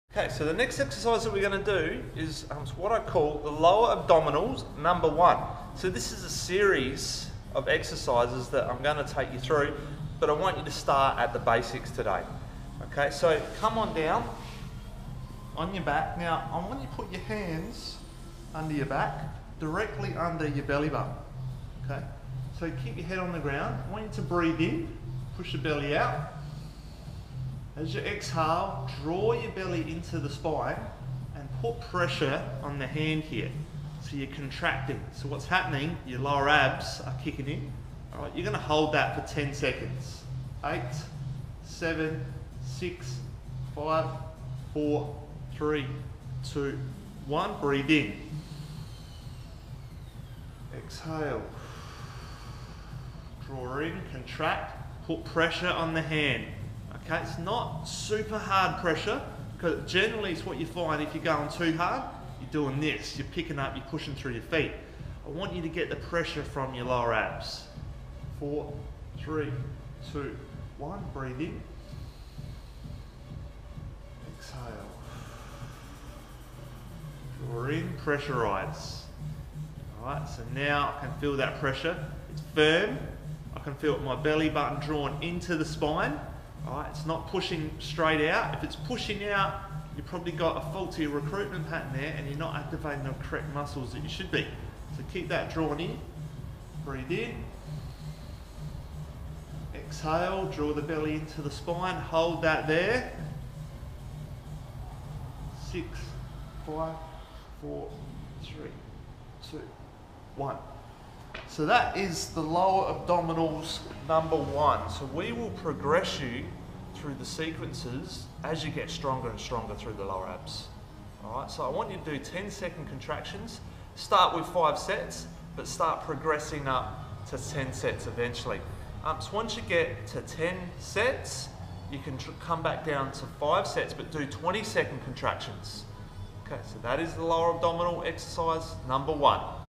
CTS lower abs #1
This is a series of core activation to rehabilitate your lower abs